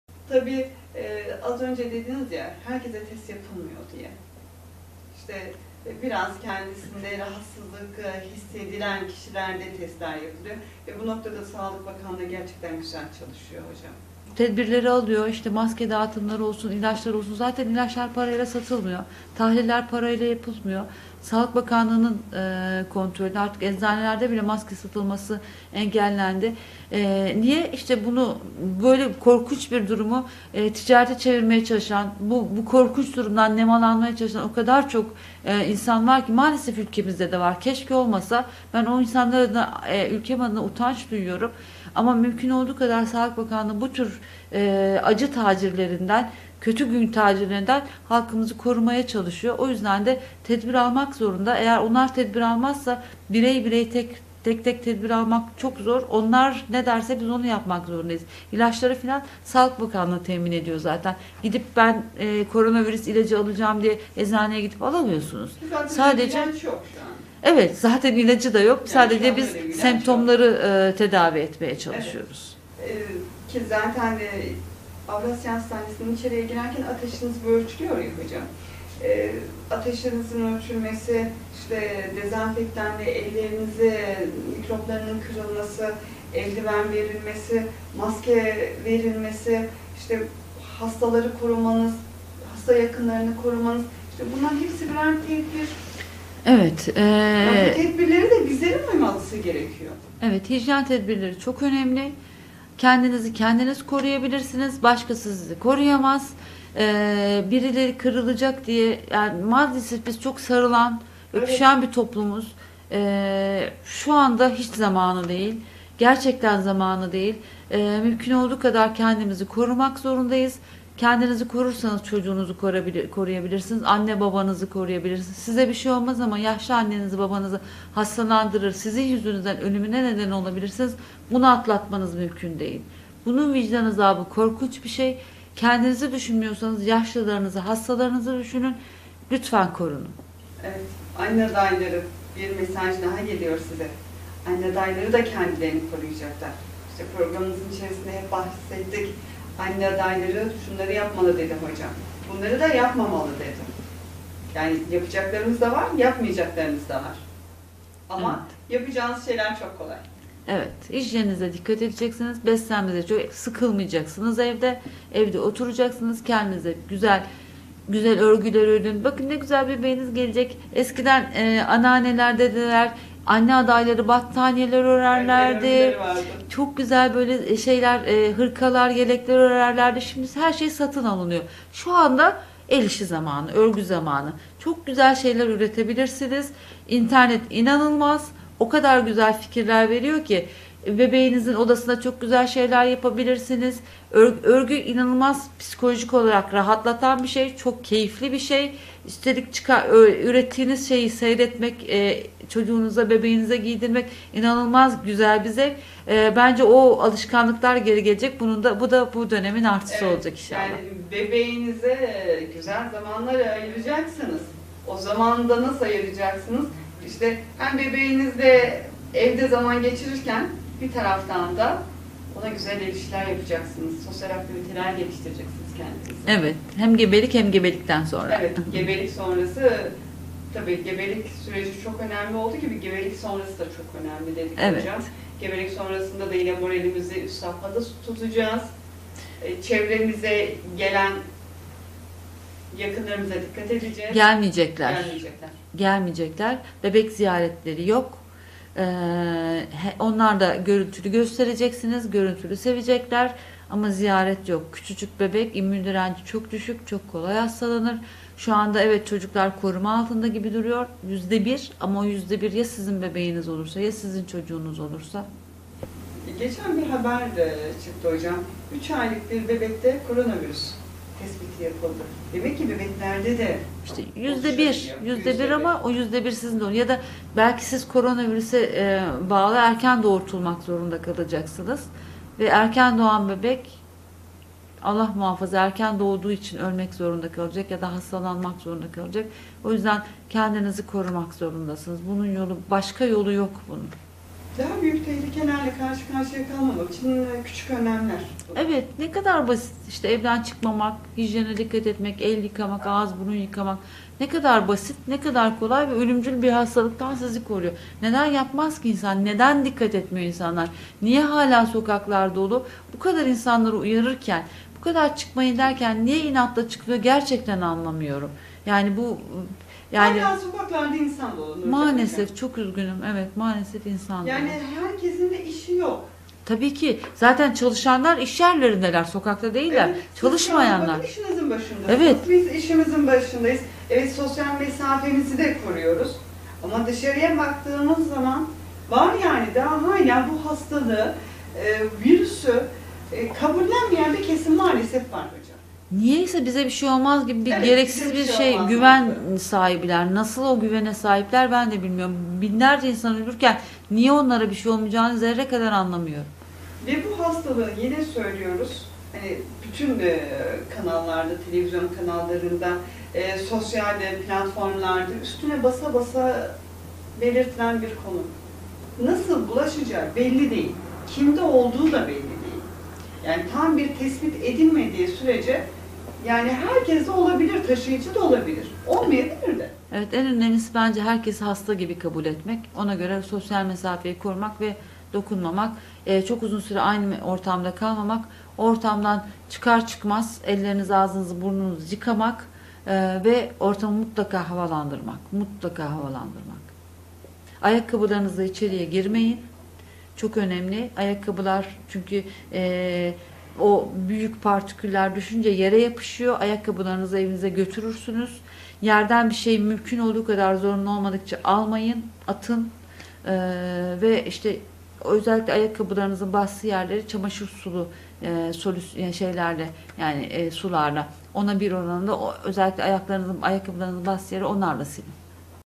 Koronavirüse Karşı Nasıl Korunmalıyız ?
Avrasya Hastanesi Çocuk Hastalıkları Uzmanı Dr. Ersin Sarı '' Koronavirüse Karşı Nasıl Korunmalıyız ? '' hakkında detaylı bilgiler veriyor.